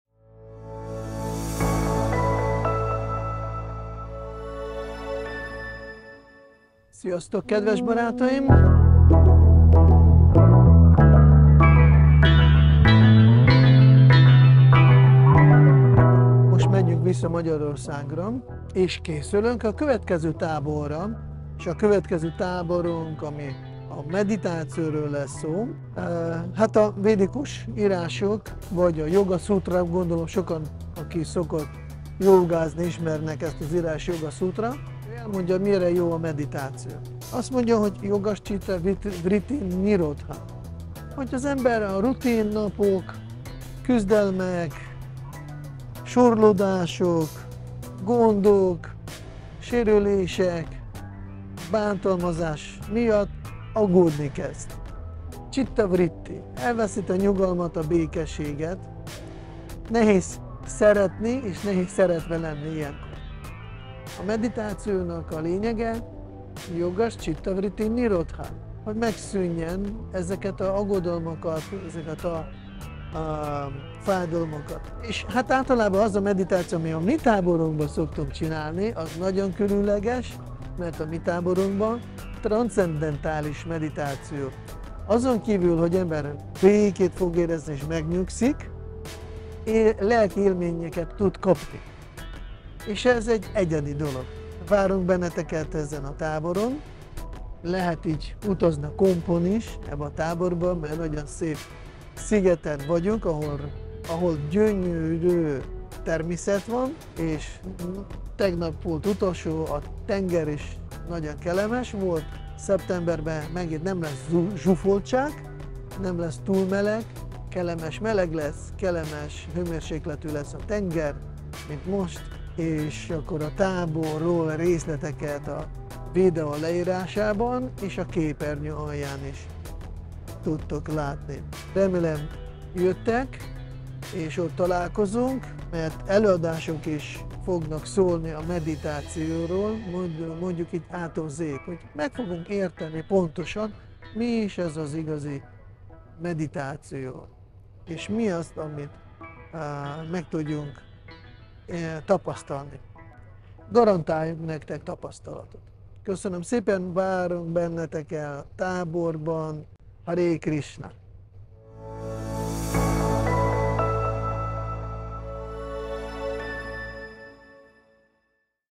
MEGHÍVÓ! Tarts Bhaktipāda Gosvāmival a Védikus Meditációs Táborba, Horvátországba!
Táborunk egy igazi különlegesség, ritka csemege a lelki fejlődésre vágyóknak. Nyaralással egybekötve ismerhetjük meg lelkünk titkait, és feltérképezhetjük további fejlődésünk útját.

Egyszer fent, máskor lent - tartja a mondás, s lássuk be, hangulatunk is így változik. Miért? Érzelmek, érzések, "lelkizés"- mindenki tudja nagyjából miről van szó, de kevesen ismerik ennek az igazi tudományát. Lehet egyáltalán tudományról beszélni, ebben a megfoghatatlannak tűnő világában? Igen! Bhaktipada Goswami az ősi indiai írások útmutatásai alapján kalauzol bennünket az értelem és érzelem valójában nagyon is átlátható birodalmában, segítve az egészséges személyiségfejlődésünket és a boldogabb mindennapokat. Táboraink helyszíne Brac szigete, az Adriai-tenger egyik legnagyobb, csodaszép természeti adottságokkal rendelkező gyöngyszeme.

Gyere el te is!

Bhaktipāda Gosvāmī